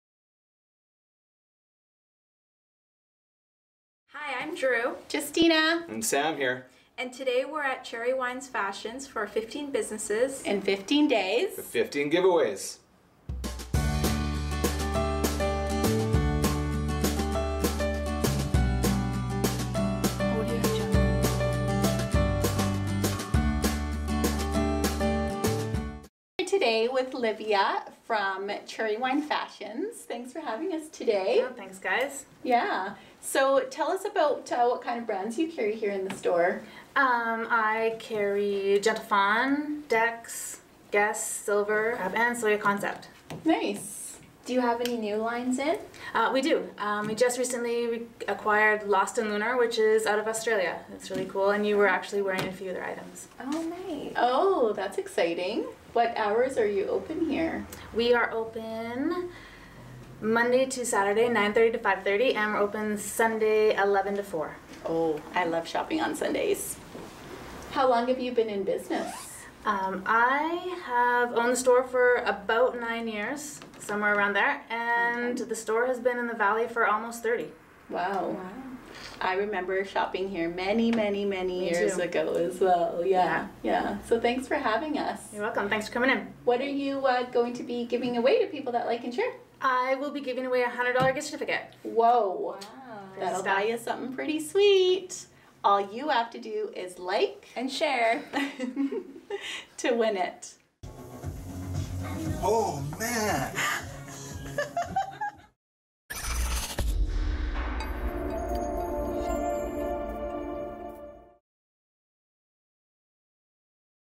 15 Businesses in 15 Days with 15 Giveaways - Day 1 Cherry Wine
DAY 1: Cherry Wine Fashions on 5th Street. Our team meets the store owner and we get to try on new clothes! 15 Businesses in 15 Days with 15 Giveaways: Join us everyday for the next 2 weeks as we visit local businesses!

Contest ends August 8th